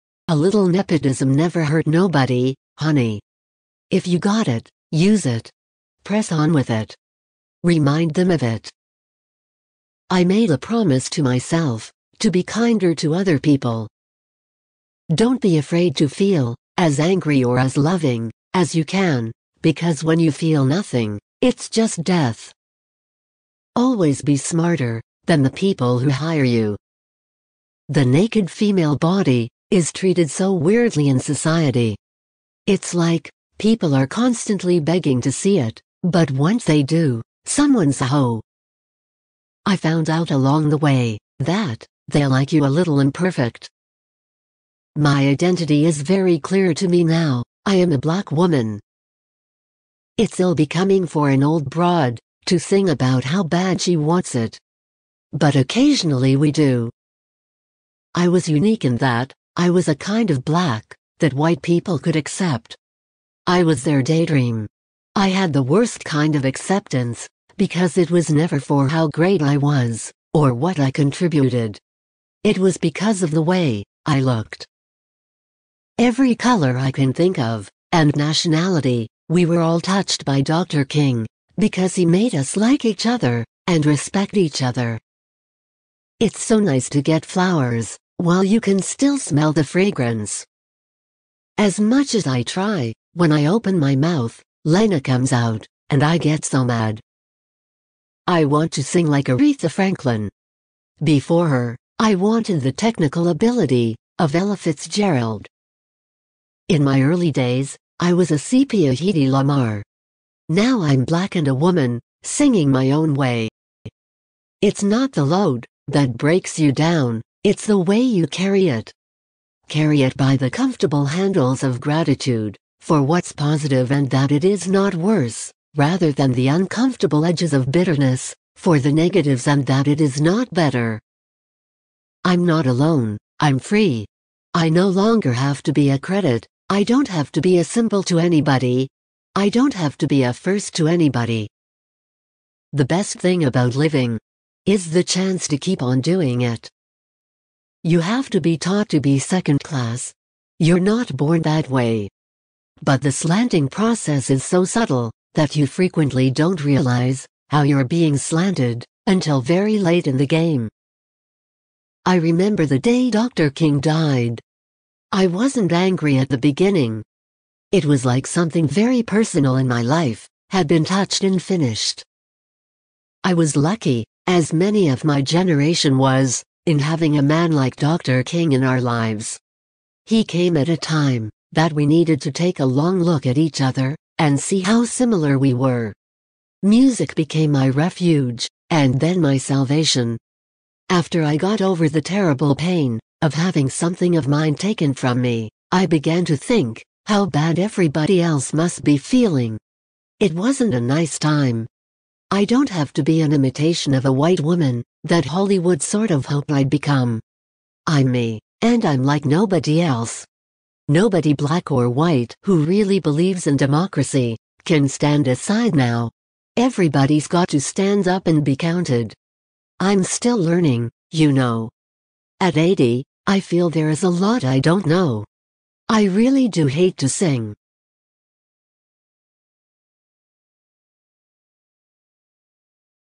Lena Horne's Inspirational Quotes for Success and Resilience | Infinite Quotes
Lena Mary Calhoun Horne (June 30, 1917 - May 9, 2010) was an American dancer, actress, singer, and civil rights activist.
 Horne's career spanned more than seventy years, appearing in film, television, and theatre.
Horne joined the chorus of the Cotton Club at the age of sixteen and became a nightclub performer before moving to Hollywood.